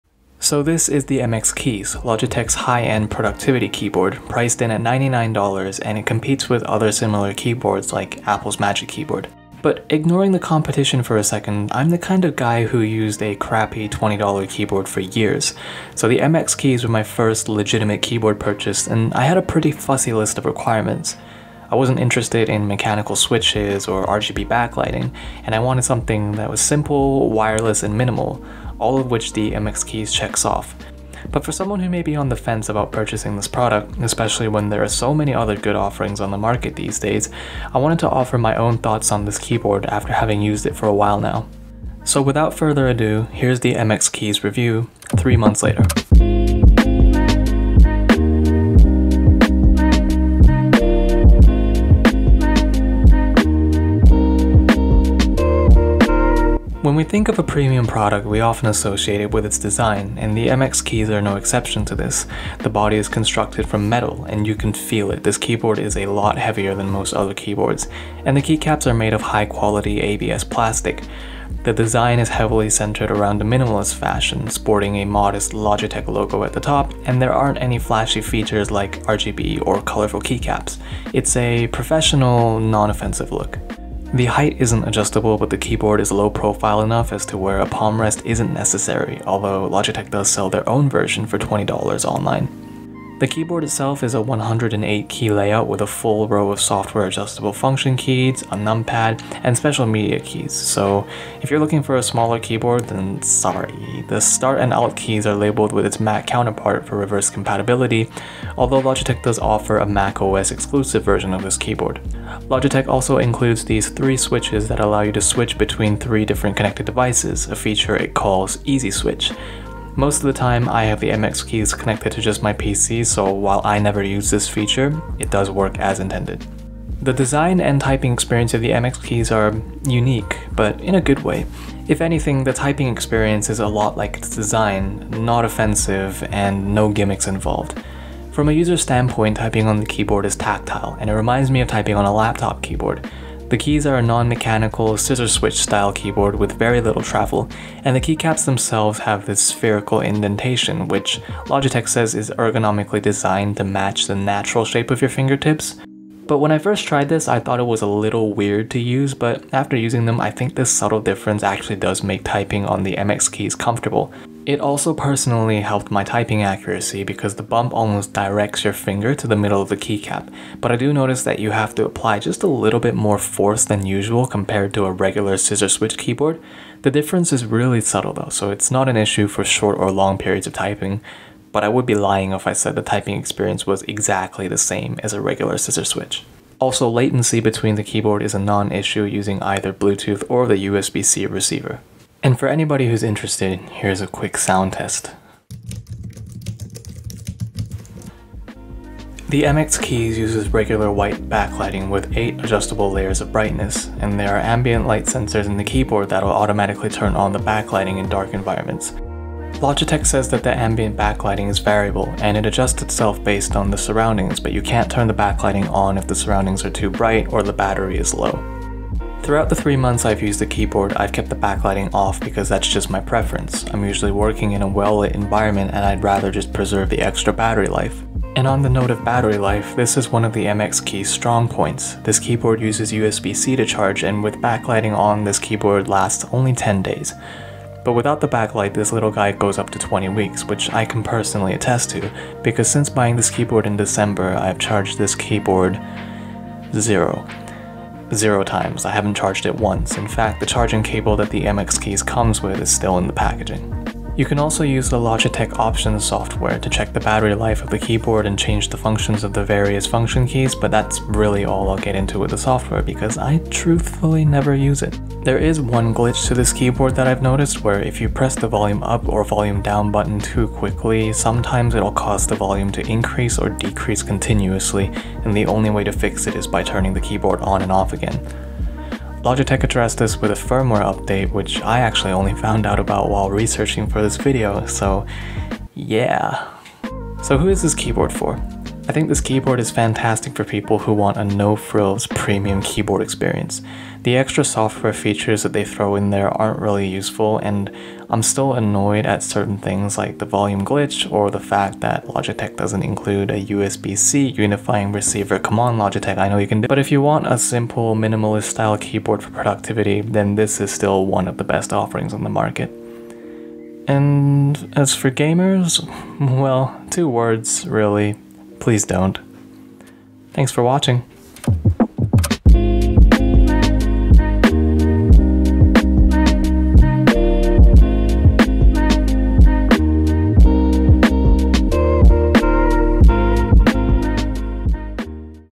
Logitech MX Keys: 3 Month Retrospective!
At $99.99 USD, the Logitech MX Keys is one of the most high-end productivity keyboards the market has to offer. But 3 months later, is that product still worth it?

Sorry for the different channel name on the video versus the current one. I'm indecisive.